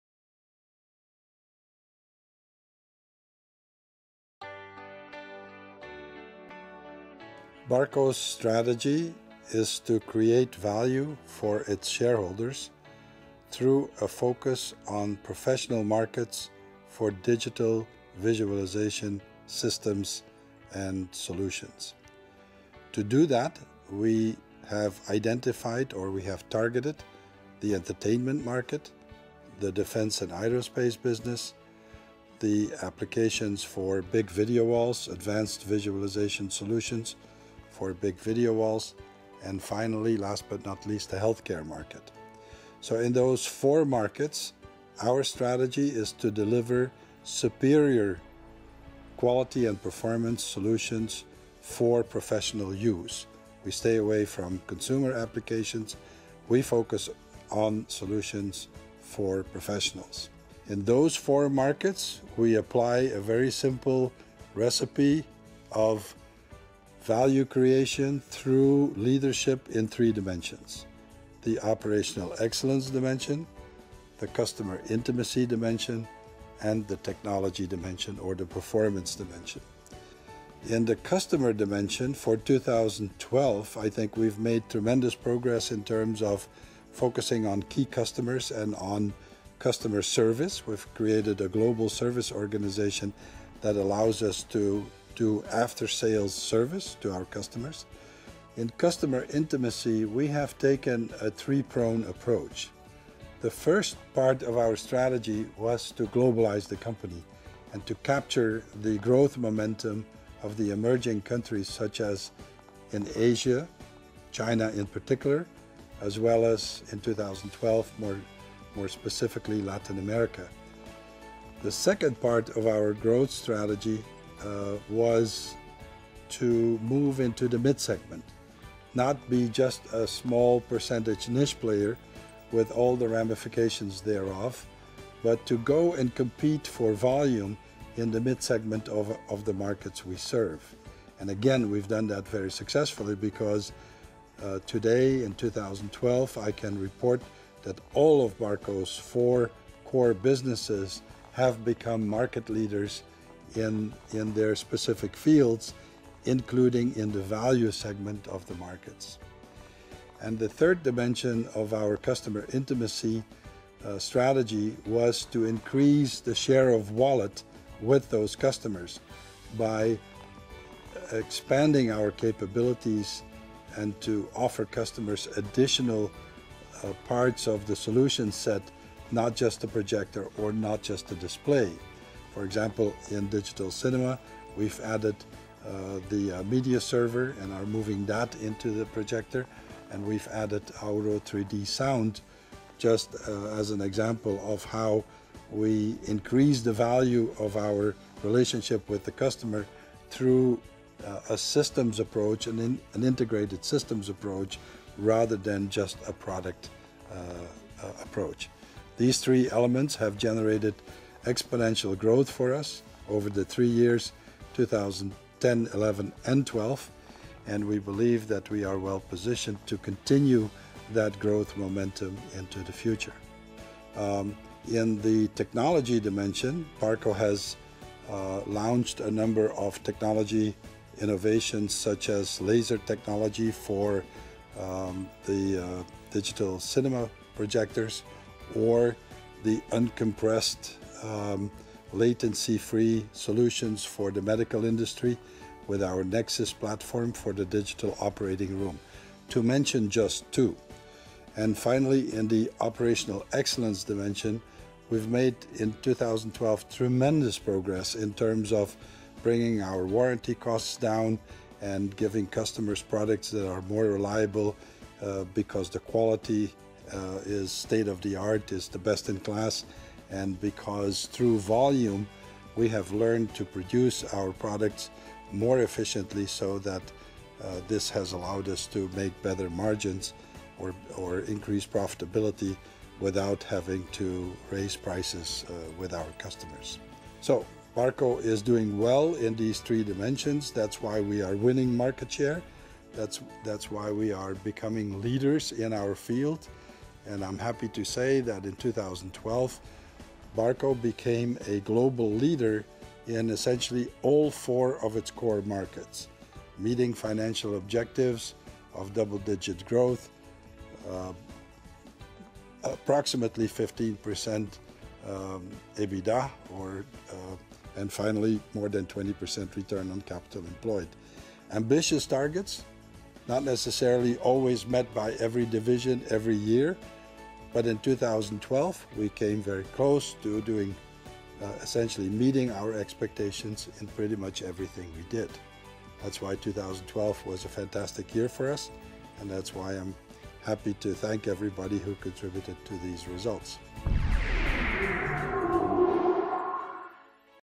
CEO Eric Van Zele discusses Barco's strategy
- Eric Van Zele, CEO of Barco, talks about the company's strategic focus on customer intimacy, technological innovation and operational excellence.